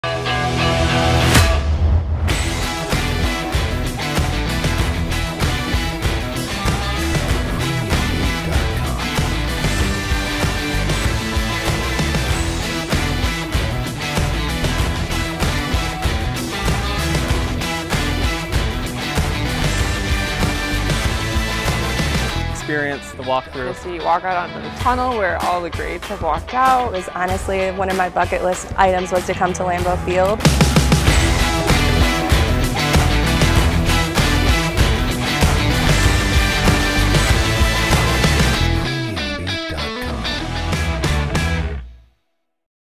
VIP Fan Experience at Lambeau Field | Packers Experiences Powered by QuintEvents
Packers Experiences powered by QuintEvents is proud to be the Official Fan Travel & Experiences Partner of the Green Bay Packers.

Our VIP Fan Experiences craft the premium gameday experience at Lambeau Field for Green Bay Packers home games, taking you closer to the action with exclusive benefits and inclusions.